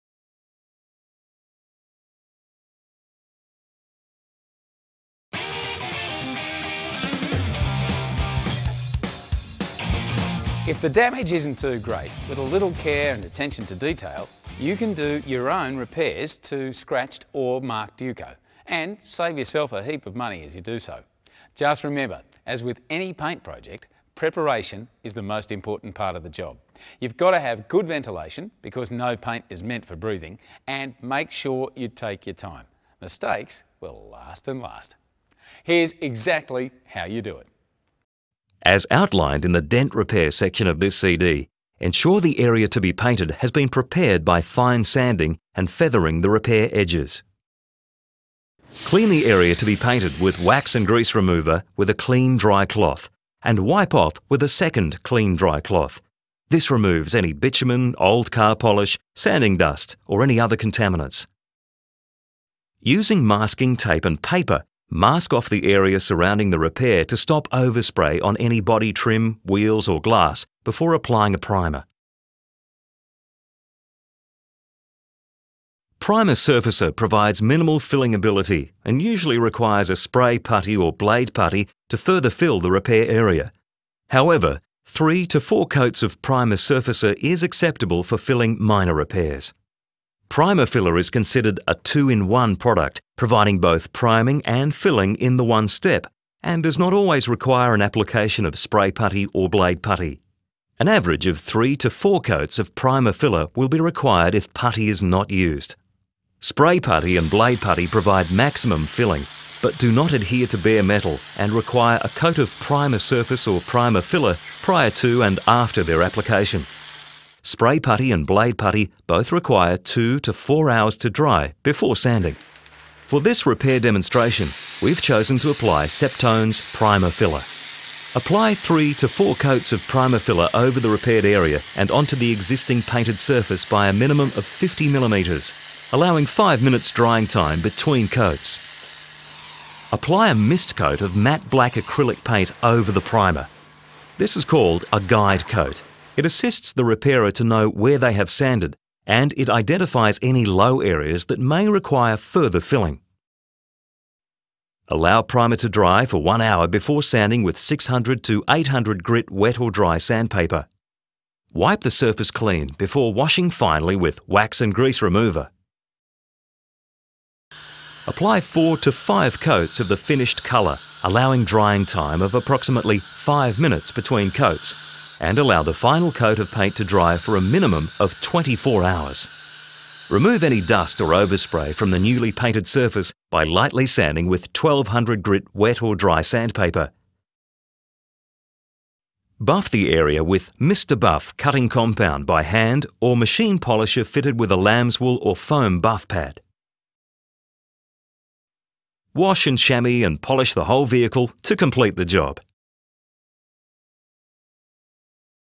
How to - Paint Repair // Supercheap Auto
With a little preparation and care, you can repair your older cars' paintwork at home for the same professional look at a fraction of the cost. By building up the colour layer-by-layer, little nicks and scratches can disappear or be almost undetectable.